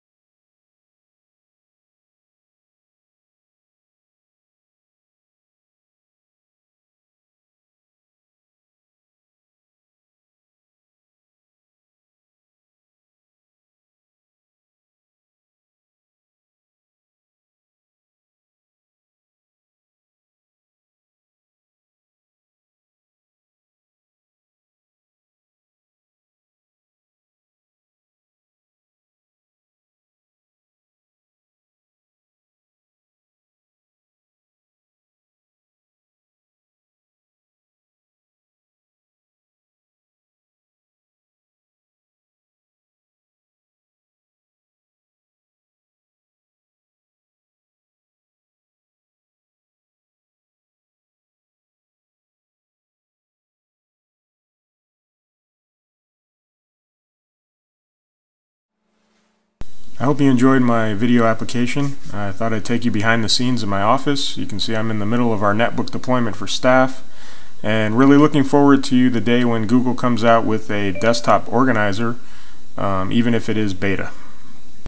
Google Teacher Academy for Administrators Application
A Technology Timeline of
Innovative Educational Leadership
In the age of Google at Le Grand Union High School District